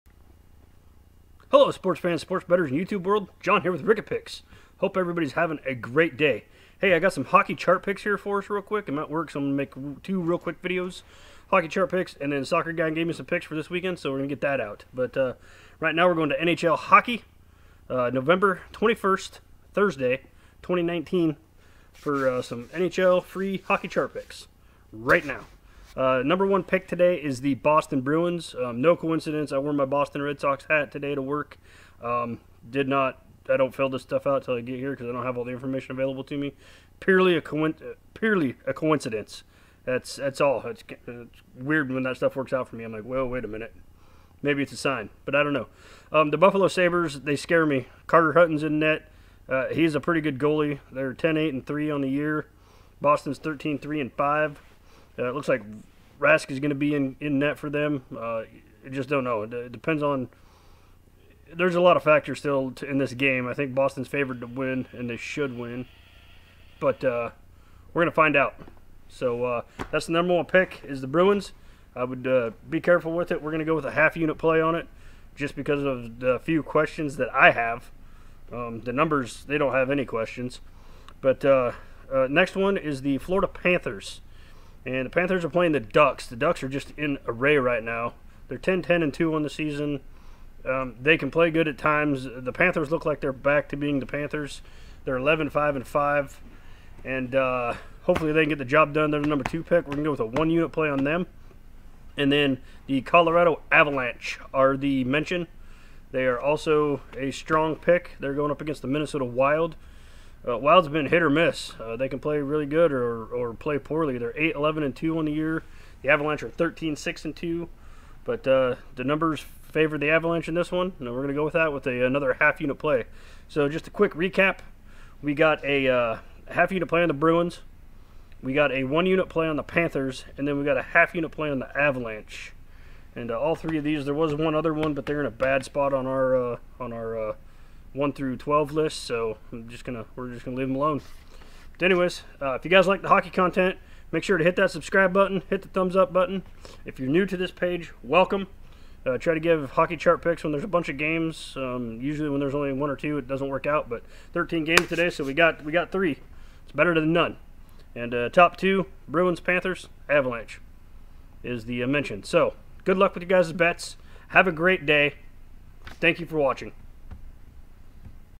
NHL Hockey Free Picks, Chart Picks, 11-21-19, Sabres @ Bruins, Ducks @ Panthers, Avalanche @ Wild
Daily sports betting advice. Hockey Chart Picks. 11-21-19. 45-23 overall. Lets crush the books.